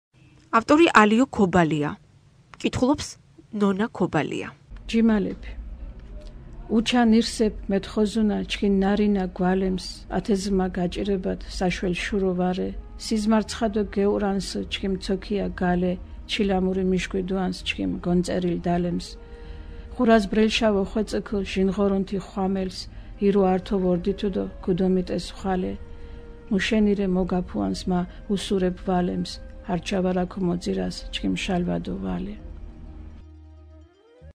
კოლხის კოლხური ლექსი 05.06.2015
ავტორი:ალიო ქობალია
კითხულობს:ნონა ქობალია